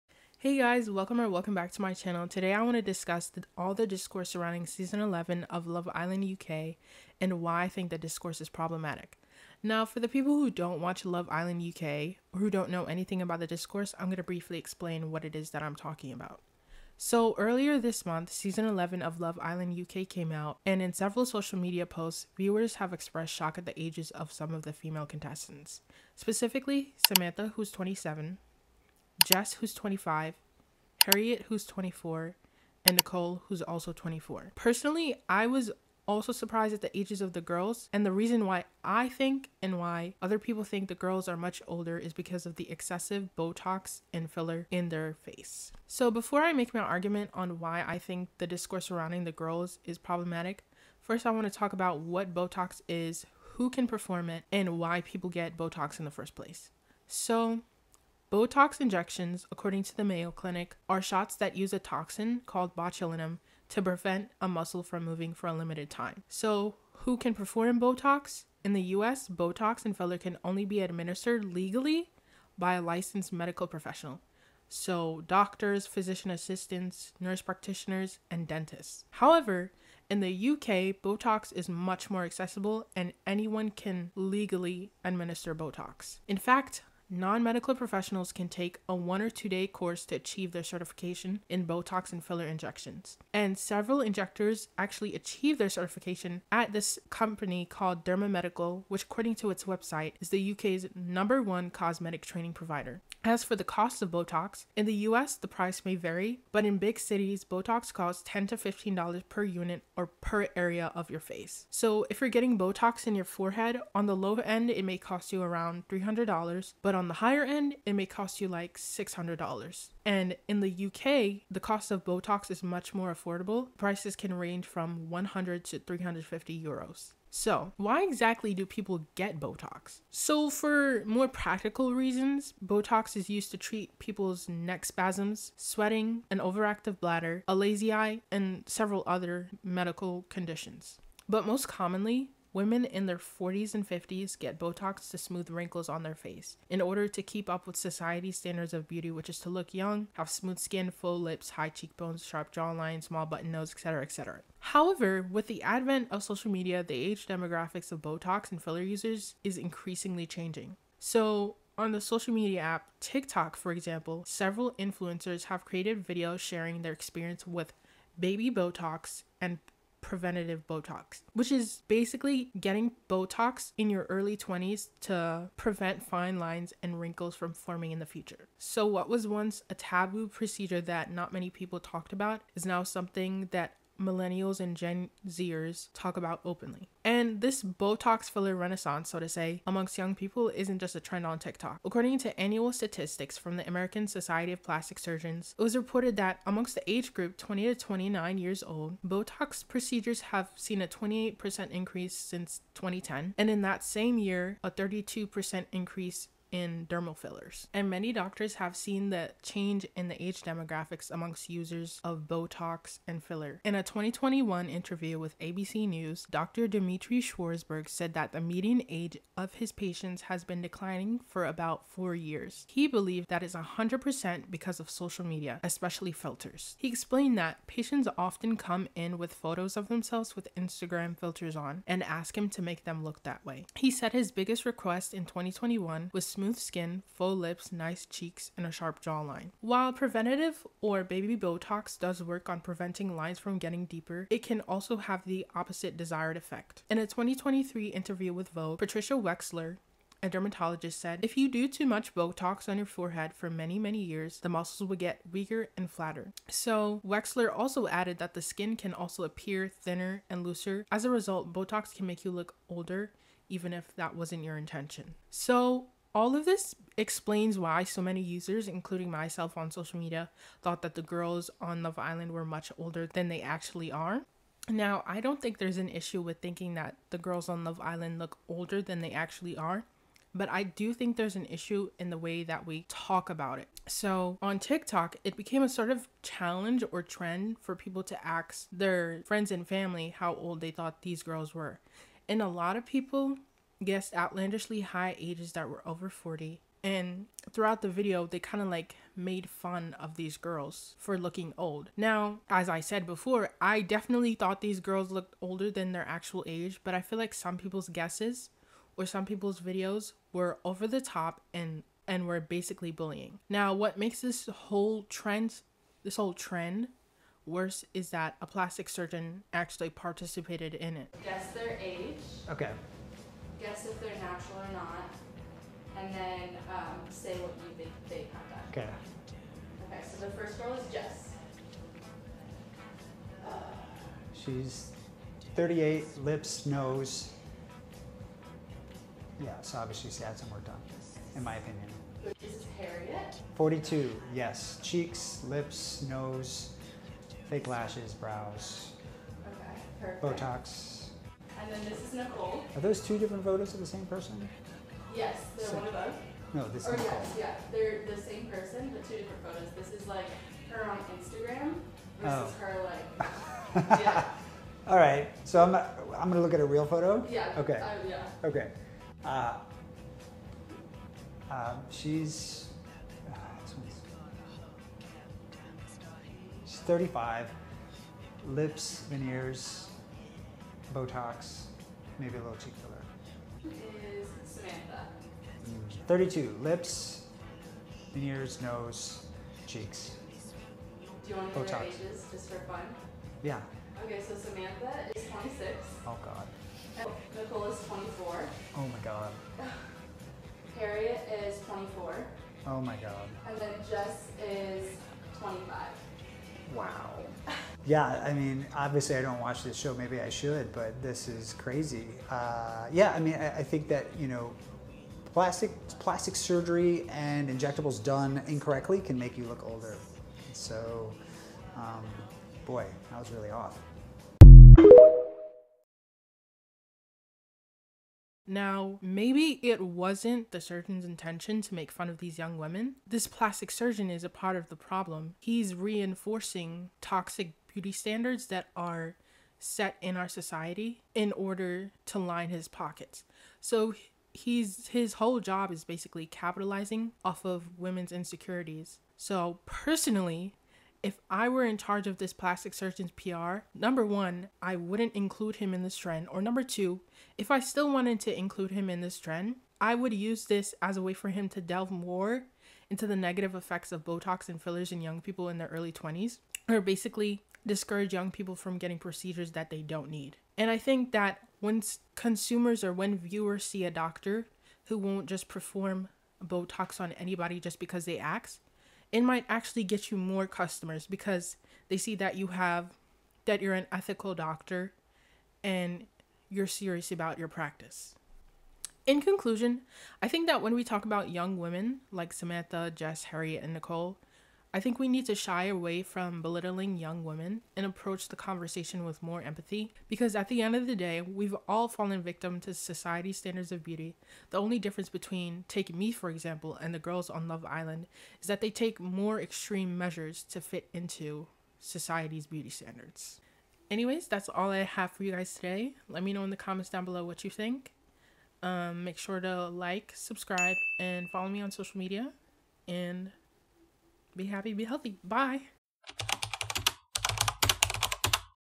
The Problem with the Love Island UK Botox Discourse
Hey guys!! Today's video is a little different than my usual content, but I hope you enjoy what I created. :)

*PLEASE make sure to be respectful in the comments down below and remember everyone is entitled to their own opinion!!